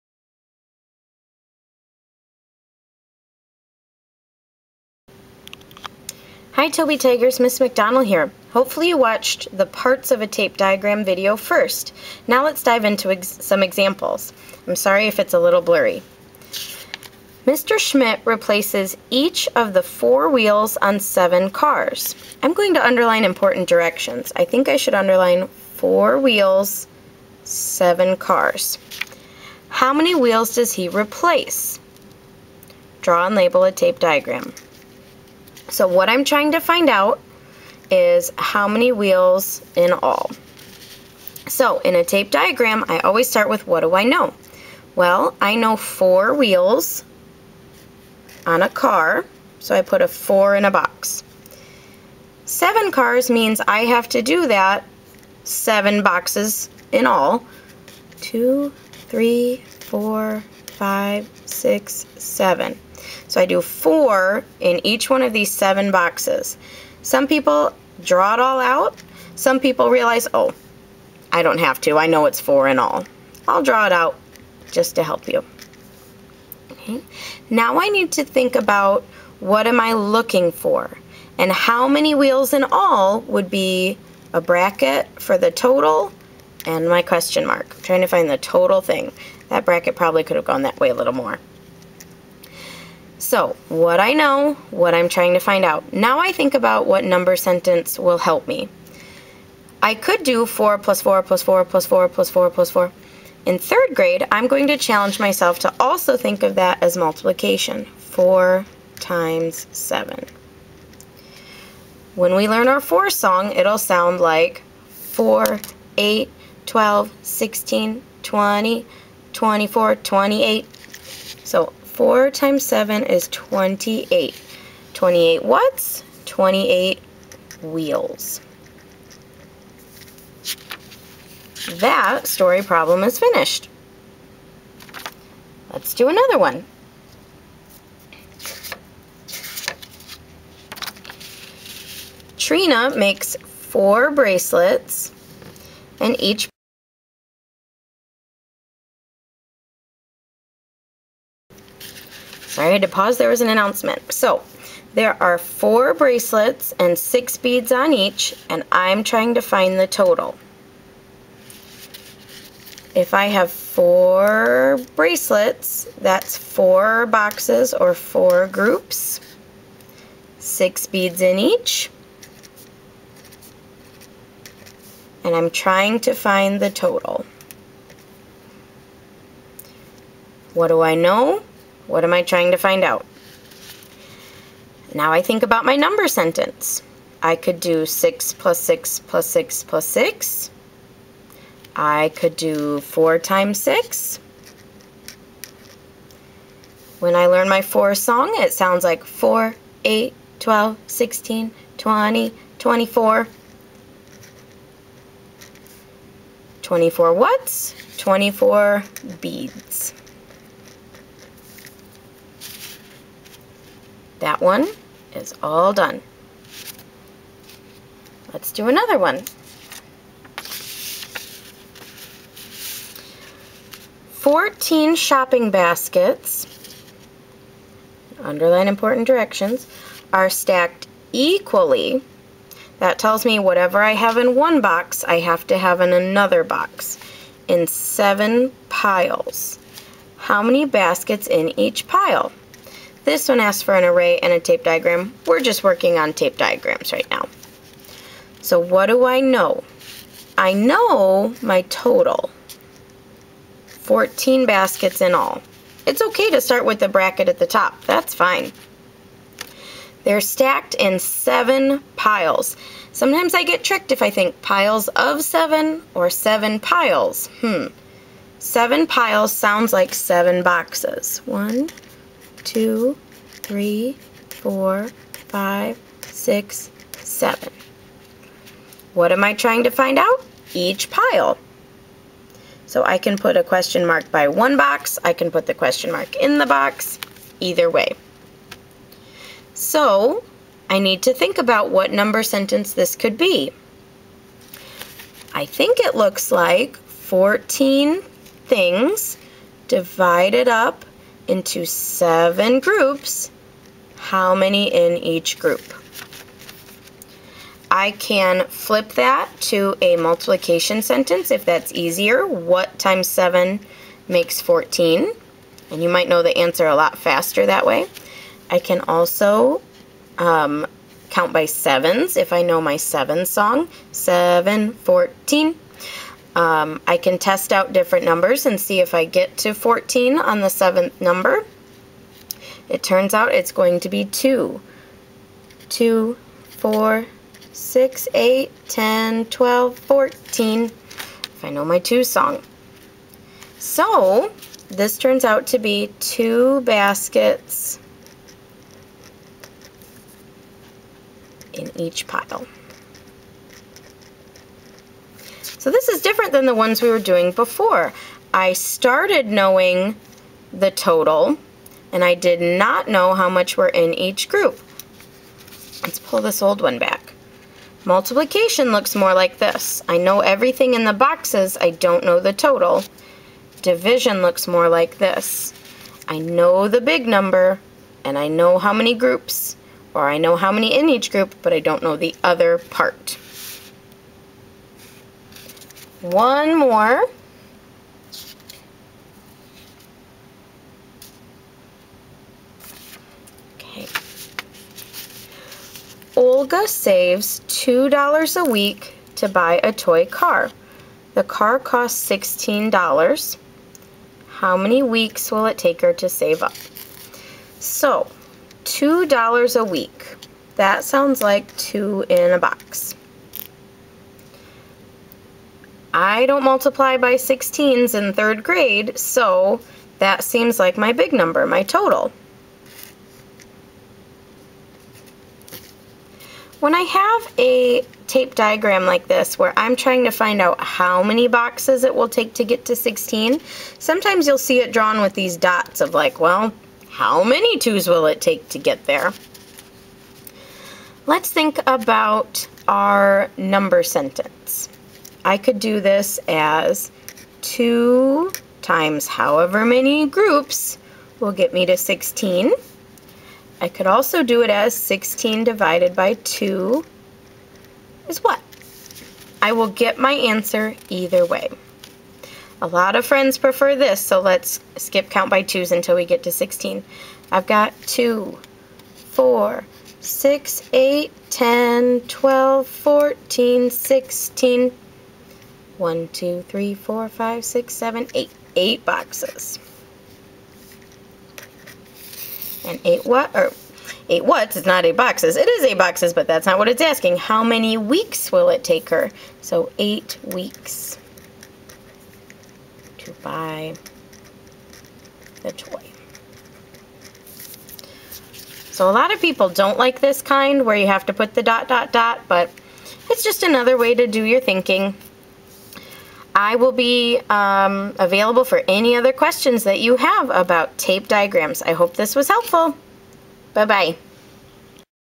Tape Diagram Examples video - 3rd grade
Make sure you watch the Parts of a Tape Diagram first (:

This video shows some example problems and the tape diagrams we could make to help us decide what number sentence would be helpful to use to solve the problem.